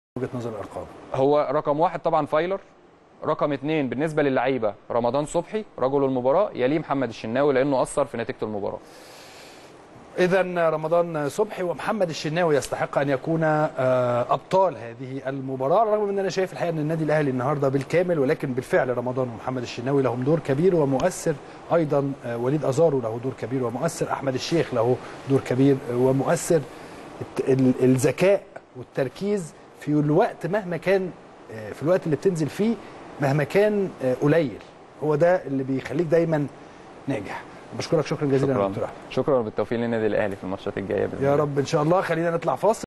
احمد ثابت يكشف عن من يستحق لقب رجل مباراة "الاهلى امام وادى دجلة"
التردد: 12341 أفقي.
القمر: نايل سات.
معامل الخطأ: 5/6.
معدل الترميز: 27500